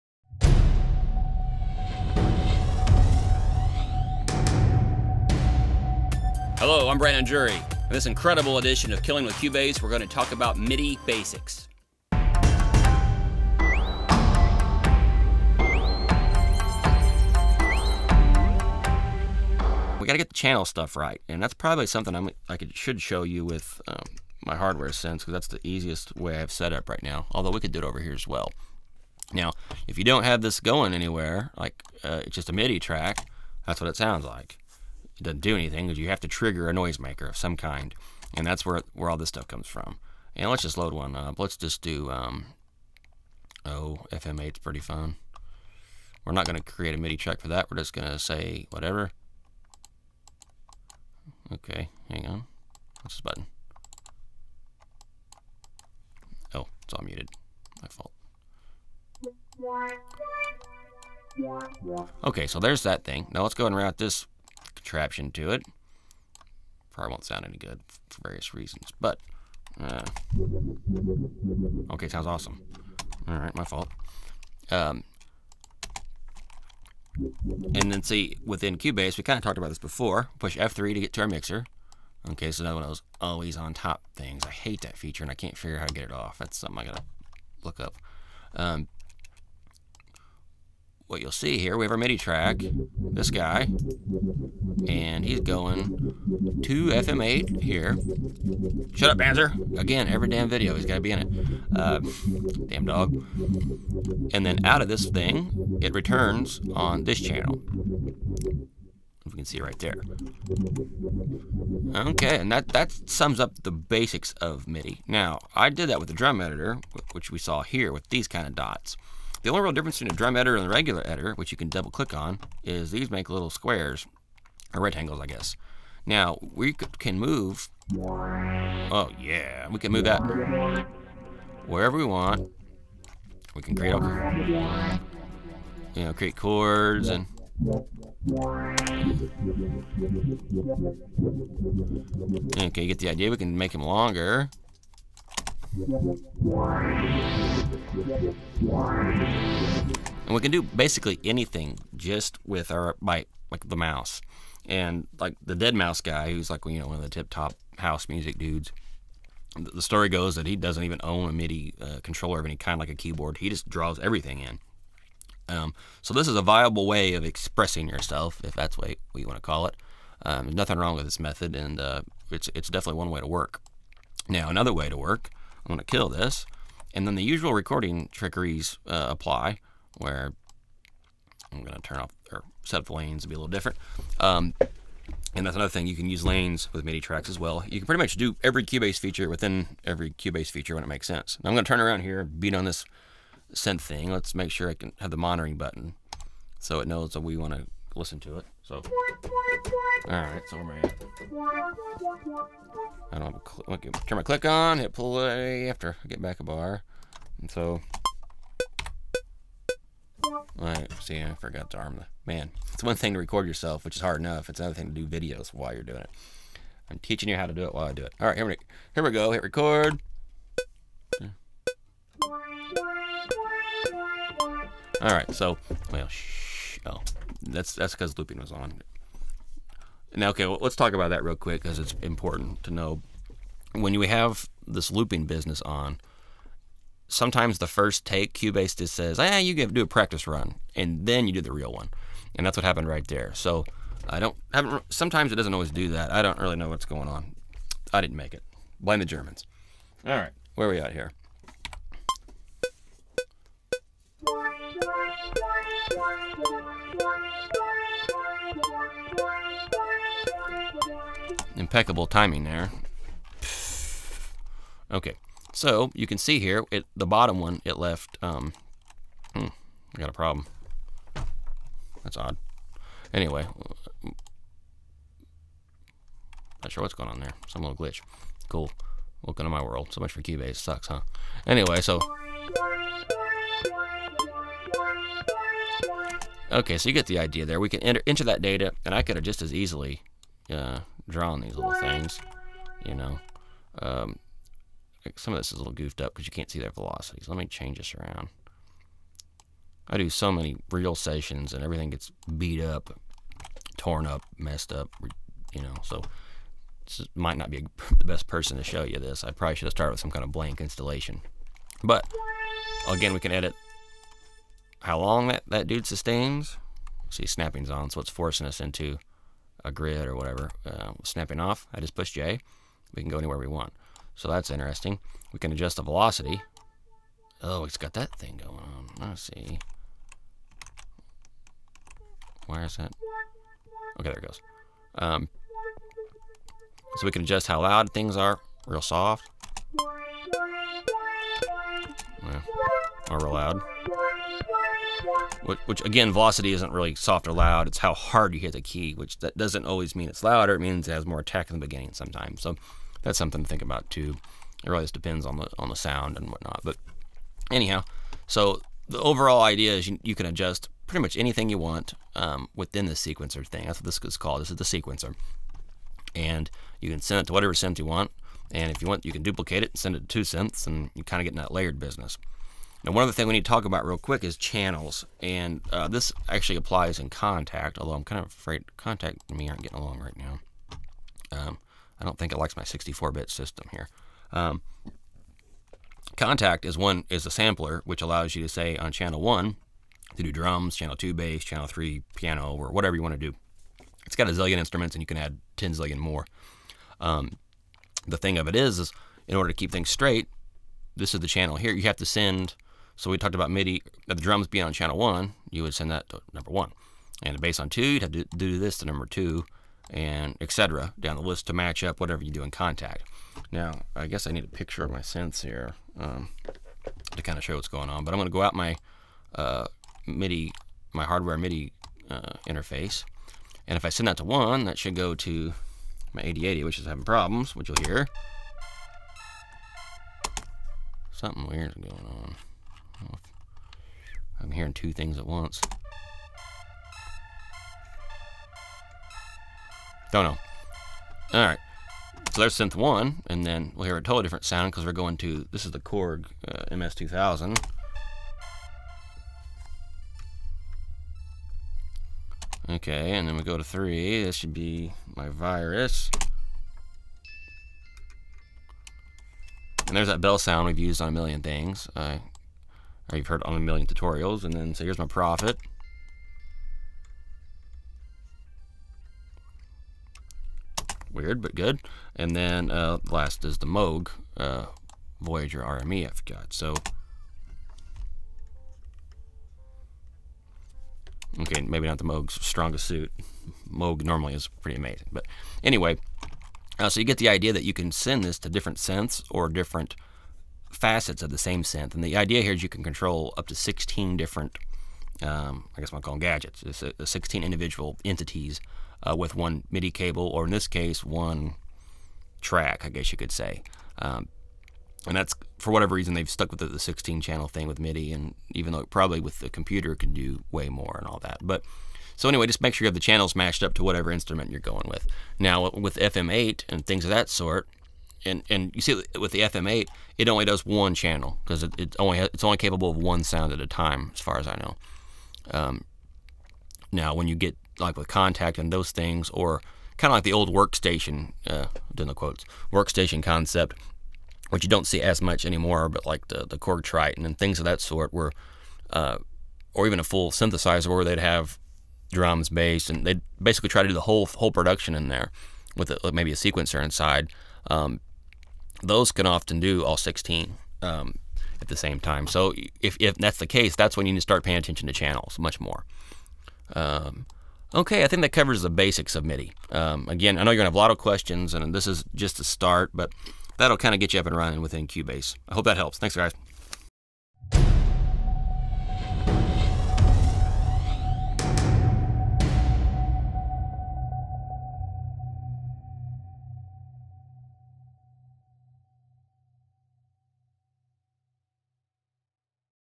016 Killing With Cubase: MIDI Basics Pt2
This is the second part to the Cubase MIDI tutorial.